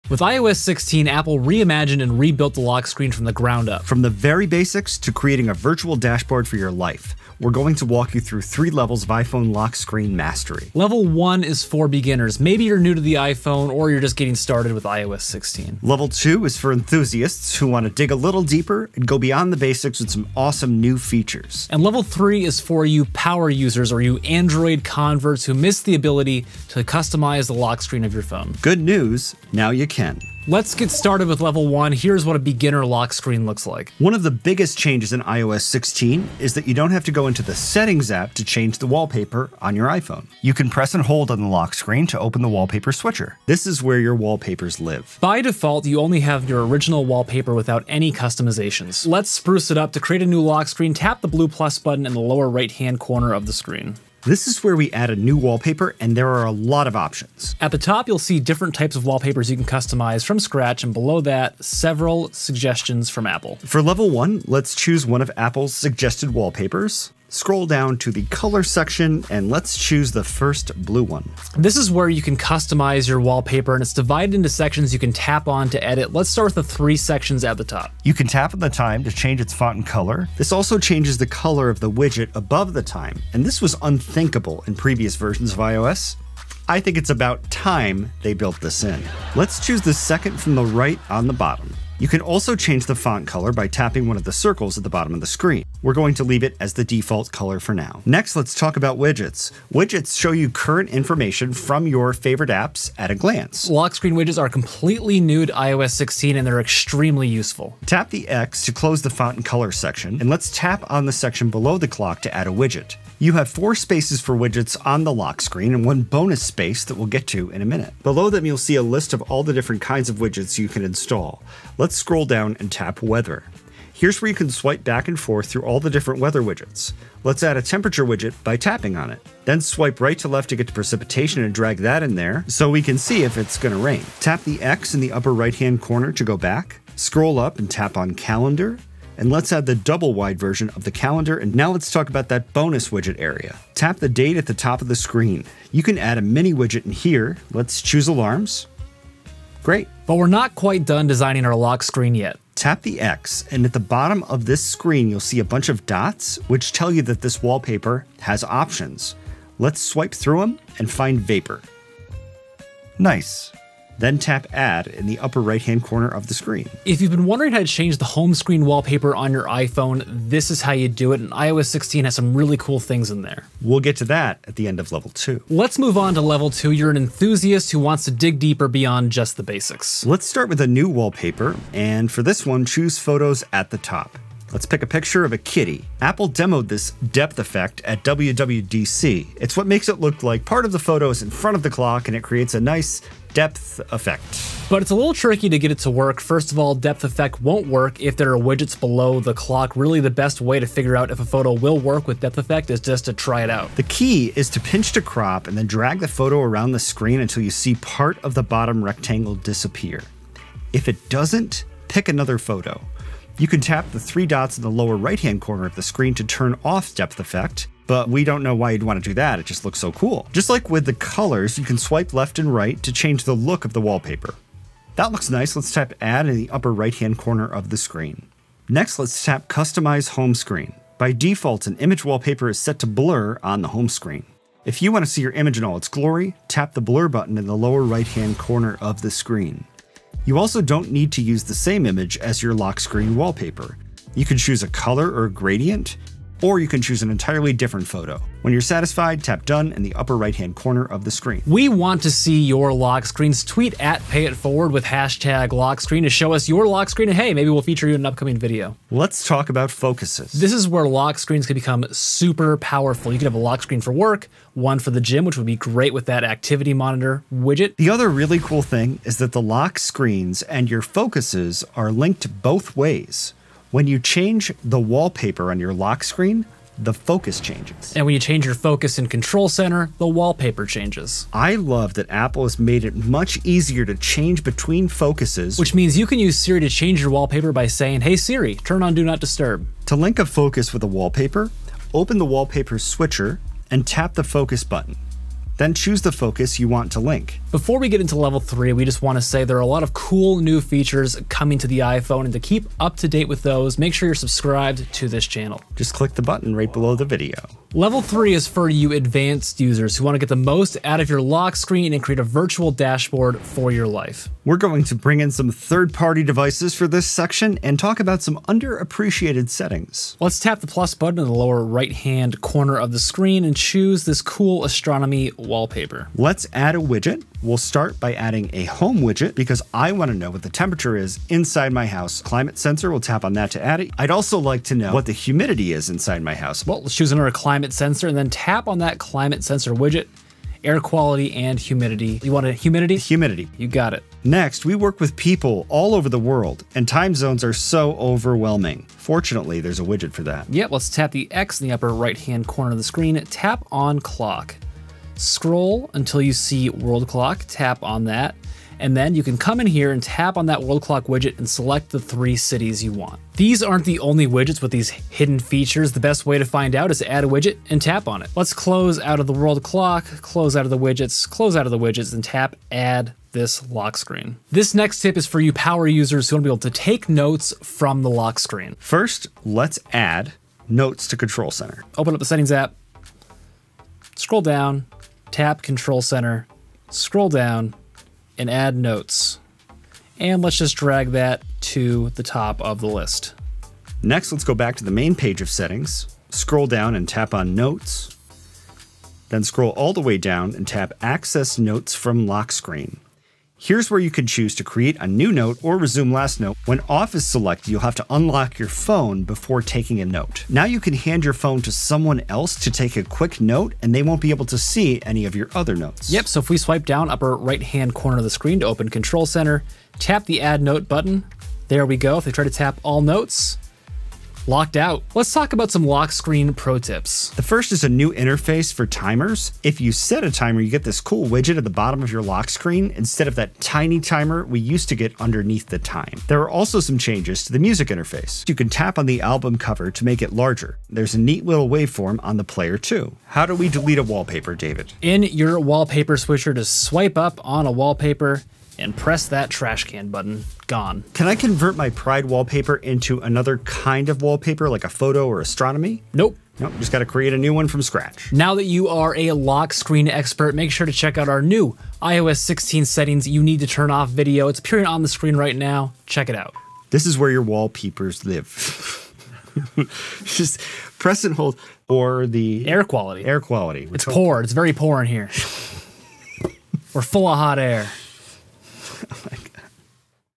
Ultimate iOS 16 Lock Screen Set Up Guide
David & David show you how to customize your iPhone lock screen with iOS 16. We'll walk you through three levels of customization — beginner, intermediate, and expert.

We'll show you how to set up wallpapers, add widgets, use Depth Effect, link Focuses, and even create a dashboard for your life!

0. Intro [0:00]
1. Level One: Beginners [0:30]
2. Level Two: Enthusiasts [2:55]
3. Level Three: Power Users [5:07]
4. How To Delete A Wallpaper [7:23]
5. Can I Convert Wallpaper Types? [7:31]